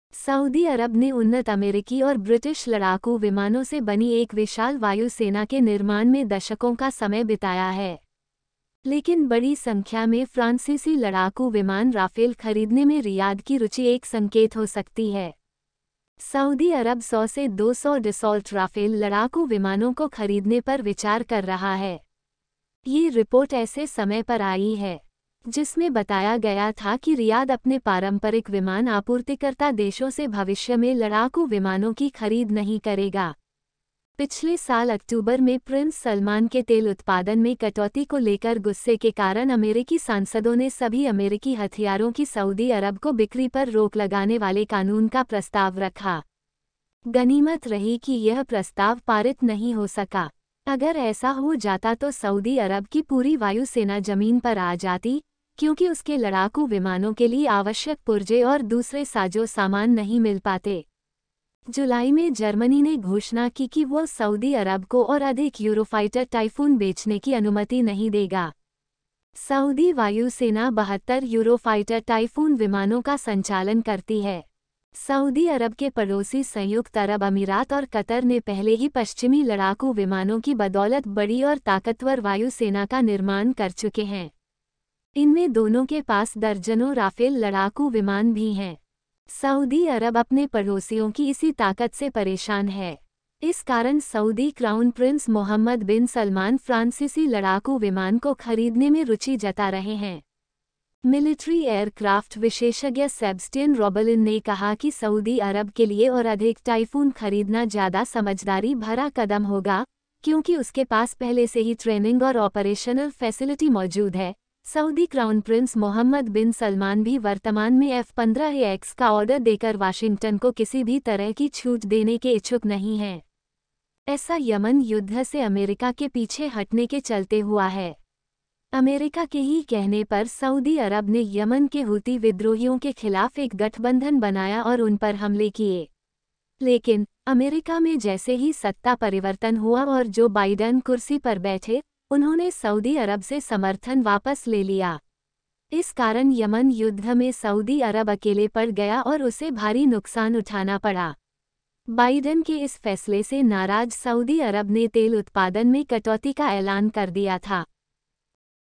😮 एक साथ 200 राफेल की डील USA के F-35 का मार्केट हजम कर लिया ! Soudi Arab will import 200 rafale
Saudi Arabia is reportedly considering a large number of French-made Dassault Rafale fighter jets. This comes almost two years after its closest ally in the region, UAE, signed a contract to buy 80 Rafales. Although not officially acknowledged by the officials in the kingdom, the reports may come as a setback to the US arms industry. Saudi Arabia has spent decades building an enormous air force composed exclusively of advanced US and British fighter jets. Riyadh’s interest in acquiring French jets may indicate that it no longer trusts its previous fighter suppliers. Watch this video to know more.